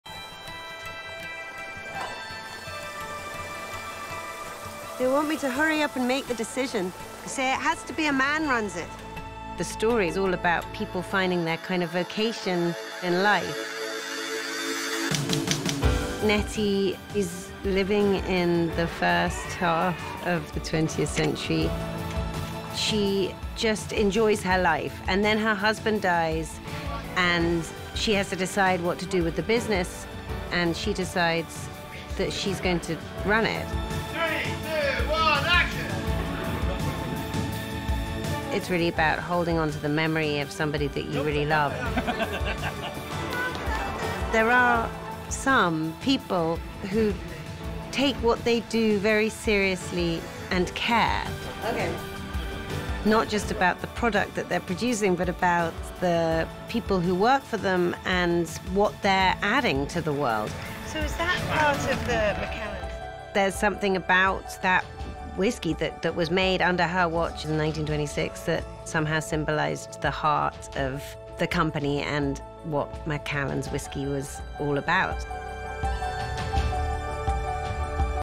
The Spirit of 1926 | In Collaboration with Emily Mortimer
The quiet heroine of The Macallan 1926 story is Janet 'Nettie' Harbinson, authentically portrayed by internationally acclaimed actor Emily Mortimer: "This story is about people finding their vocation in life. There’s something about that whisky that was made under her watch in 1926 that symbolised the heart of the company and what The Macallan’s whisky was all about." A sentiment that we echo and one that enables us to continue to enrich the lives of the community that surrounds us today.

Crafted without compromise. Please savour The Macallan responsibly.